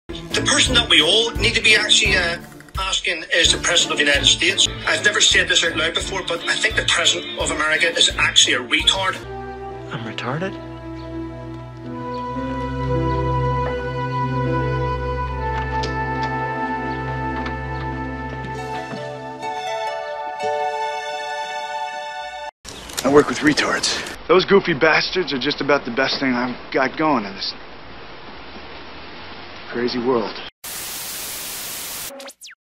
People are asking if Joe Biden is a "retard"
Hate to insult the mentally challenged and the amazing people that take care of them! The public is asking if Joe Biden might actually be "retarded".